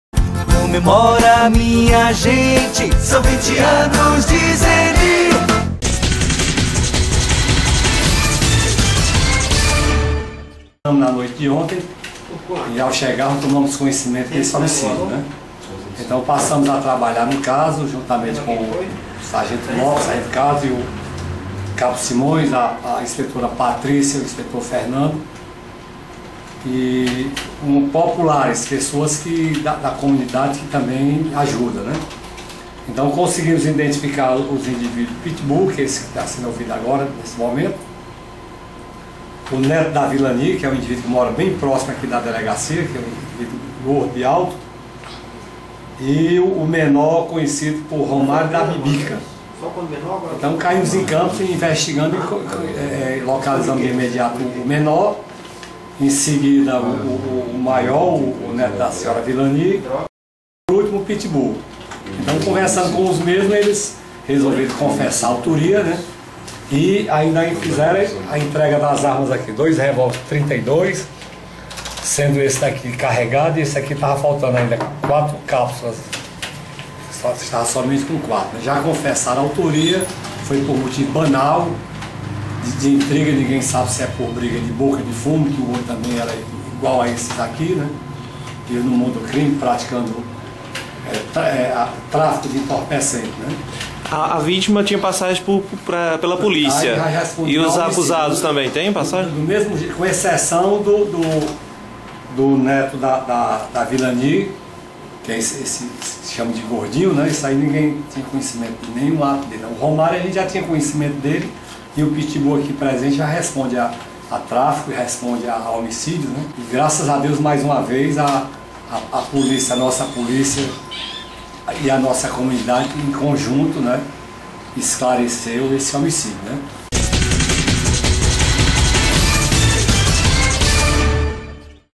Polícia apreende três acusados de envolvimento no homicídio da "7 de Setembro" em Iguatu
A polícia civil de Iguatu, na manhã desta terça-feira, 03, através de seu serviço de inteligência prendeu três acusados de terem participado do homicídio contra o vendedor de mel, Weslei Lopes de Lima 24 anos, na madrugada do último sábado, 30, na Rua 7 de setembro em Iguatu.